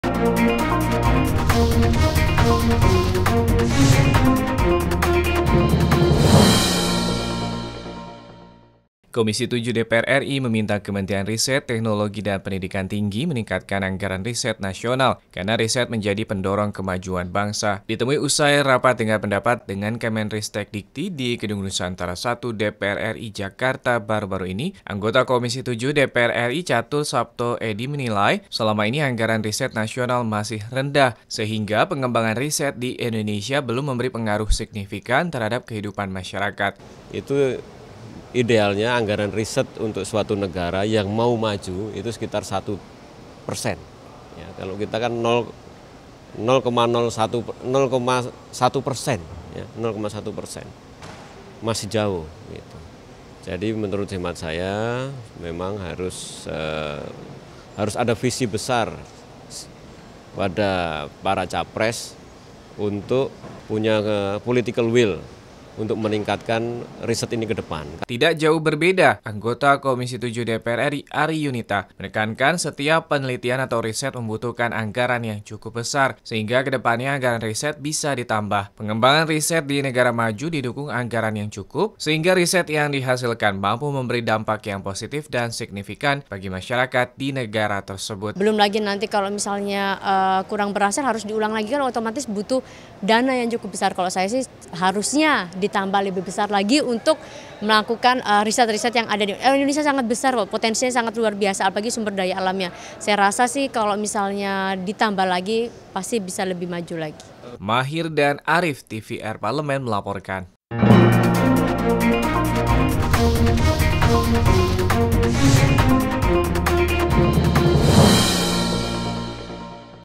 DPR RI - DPR RI - KOMISI VII DORONG PEMERINTAH TINGKATKAN ANGGARAN RISET
NARSUM :
TJATUR SAPTO EDY / Anggota Komisi VII DPR RI / FPAN / Dapil JATENG VI
ARI YUSNITA / Anggota Komisi VII DPR RI / FPNASDEM / Dapil KALTIM